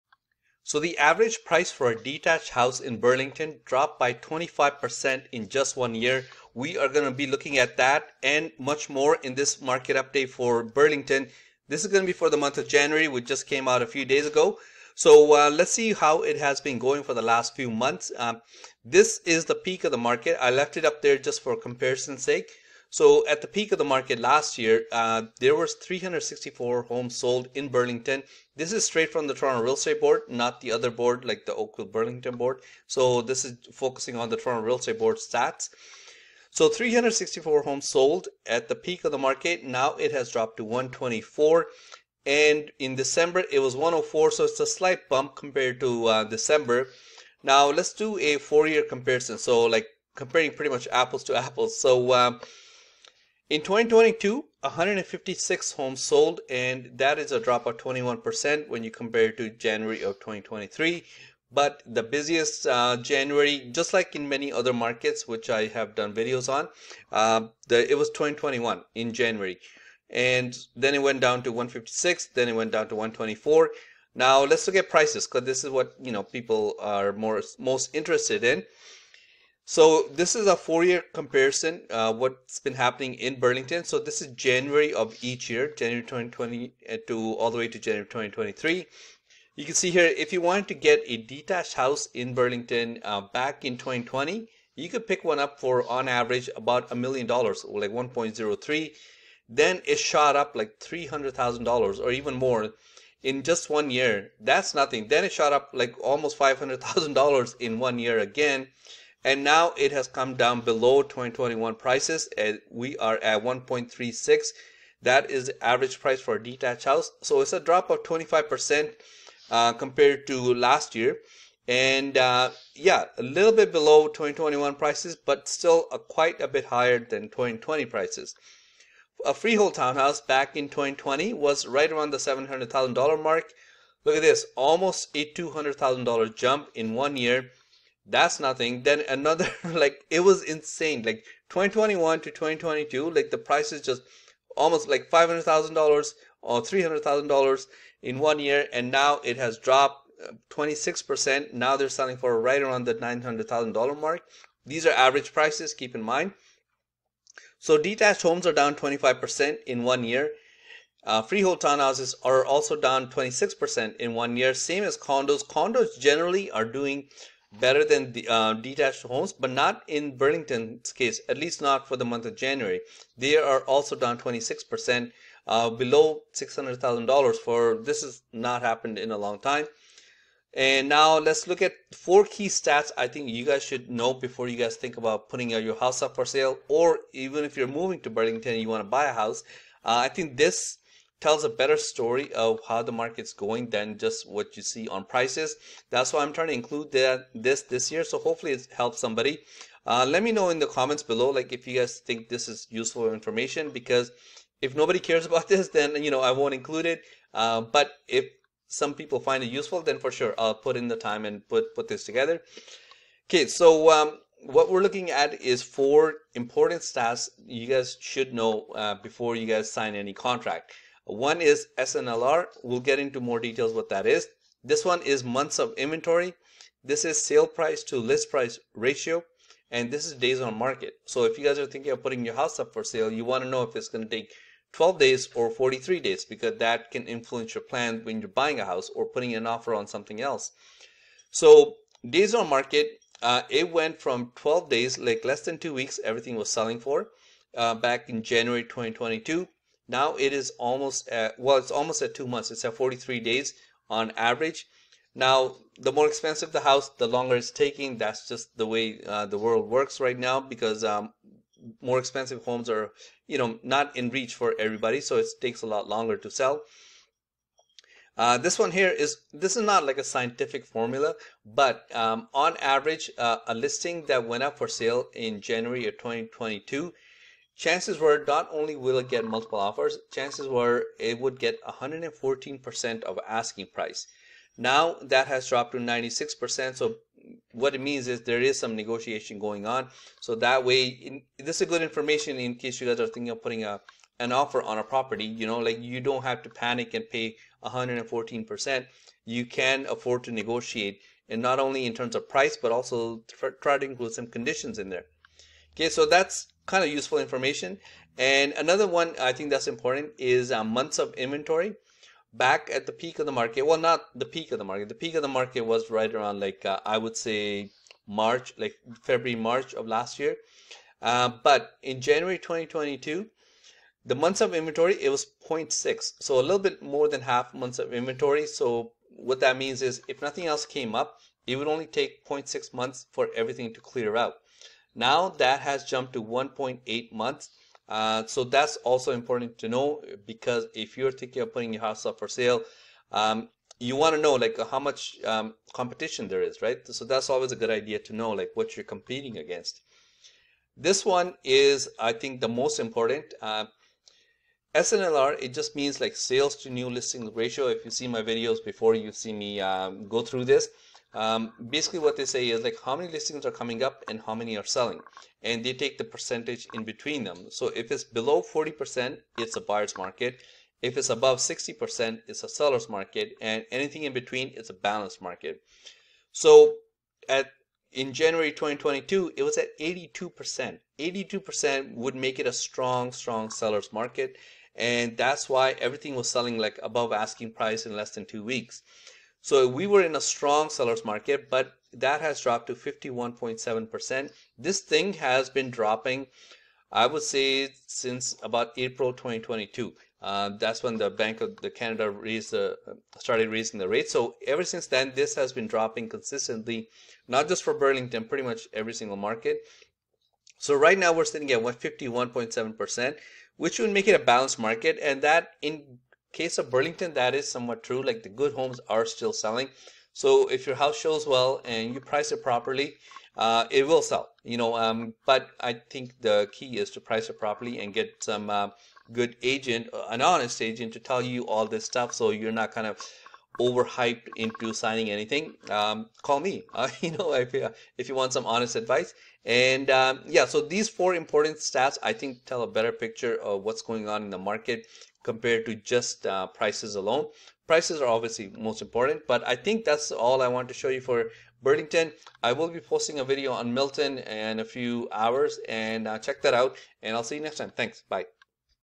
Burlington Real Estate Update: Detached Home Prices PLUMMET 25% in 1 Year!
Burlington real estate prices did not have a strong start to the year 2023. Burlington detached homes dropped 25% in just 1 year. See prices for Burlington detached homes, freehold townhouses and Burlington condos.

Moving to Burlington Ontario? Watch this market update to know exactly how the real estate market is doing here. No sales bias.

Vic Singh
Realtor®
Cloud Realty Brokerage